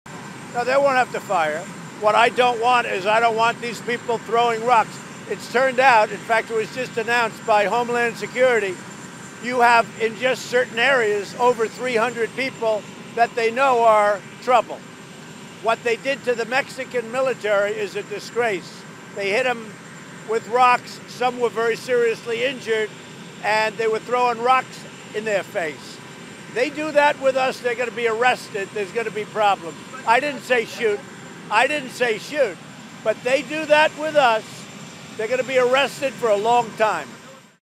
Trump says troops won't shoot migrants at border
President Donald Trump says if migrants throw rocks at US troops they're not going to be shot, but they're "going to be arrested." He previously had said the US military should react to rock-throwing migrants as though the rocks were "rifles." (Nov. 2) -- The Associated Press